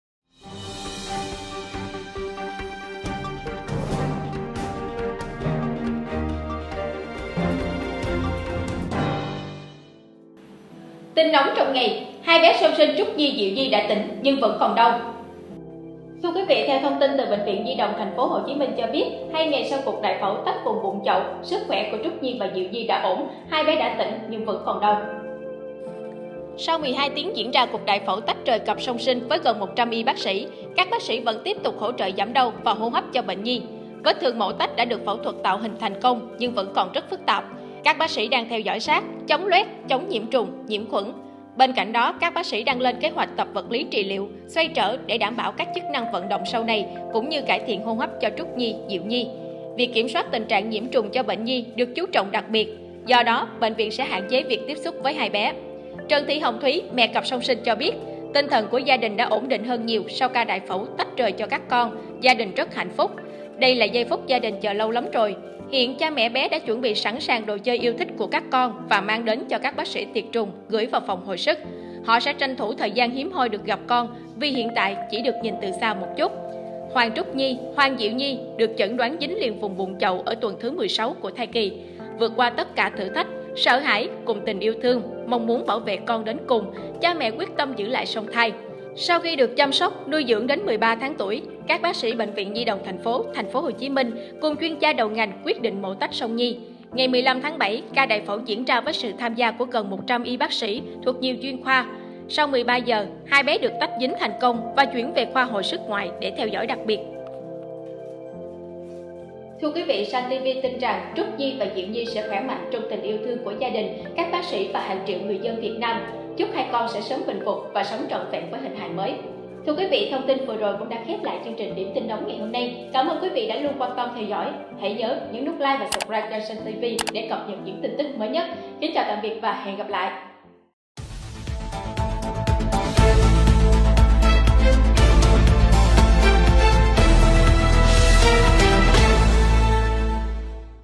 2 bé song sinh Trúc Nhi – Diệu Nhi đã tỉnh, nhưng vẫn còn đau | SANtv
Sau 12h tiếng diễn ra cuộc đại phẫu tách rời cặp đôi song sinh với gần 100 y bác sĩ, các bác sĩ vẫn tiếp tục hỗ trợ giảm đau và hô hấp cho bệnh nhi. Vết thương mổ tách đã được phẫu thuật tạo hình thành công nhưng còn rất phức tạp. Các bác sĩ đang theo dõi sát, chống loét, chống nhiễm trùng, nhiễm khuẩn. Bên cạnh đó, các bác sĩ đang lên kế hoạch tập vật lý trị liệu, xoay trở để đảm bảo các chức năng vận động sau này, cũng như cải thiện hô hấp cho Trúc Nhi - Diệu Nhi. Việc kiểm soát tình trạng nhiễm trùng cho bệnh nhi được chú trọng đặc biệt. Do đó, bệnh viện sẽ hạn chế việc tiếp xúc với hai bé.
Trần Thị Hồng Thuý (mẹ cặp song sinh) cho biết, tinh thần của gia đình đã ổn định hơn nhiều sau ca đại phẫu tách rời cho các con, gia đình rất hạnh phúc. Đây là giây phút gia đình chờ lâu lắm rồi. Hiện, cha mẹ bé đã chuẩn bị sẵn đồ chơi yêu thích của các con và mang đến cho bác sĩ tiệt trùng, gửi vào phòng hồi sức. Họ sẽ tranh thủ thời gian hiếm hoi được gặp con vì hiện tại chỉ được nhìn từ xa một chút
Hoàng Trúc Nhi - Hoàng Diệu Nhi được chẩn đoán dính liền bụng chậu ở tuần thứ 16 của thai kỳ. Vượt qua tất cả thử thách, sợ hãi, cùng tình yêu thương, mong muốn bảo vệ con đến cùng, cha mẹ quyết tâm giữ lại song thai. Sau khi được chăm sóc, nuôi dưỡng đến 13 tháng tuổi, các bác sĩ Bệnh viện Nhi đồng Thành phố (TP.HCM) cùng chuyên gia đầu ngành quyết định mổ tách song Nhi. Ngày 15/7, ca đại phẫu diễn ra với sự tham gia của gần 100 y bác sĩ thuộc nhiều chuyên khoa. Sau 13 giờ, hai bé được tách dính thành công và chuyển về khoa Hồi sức Ngoại để theo dõi đặc biệt.